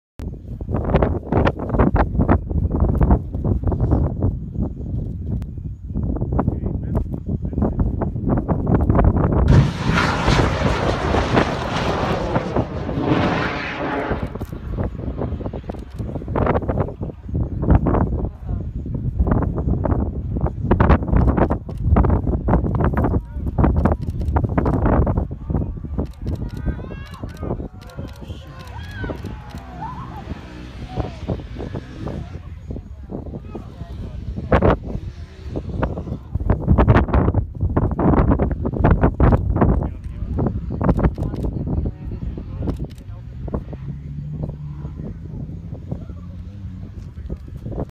Flat earther' dies in rocket crash
'Mad Mike' Hughes killed in homemade rocket crash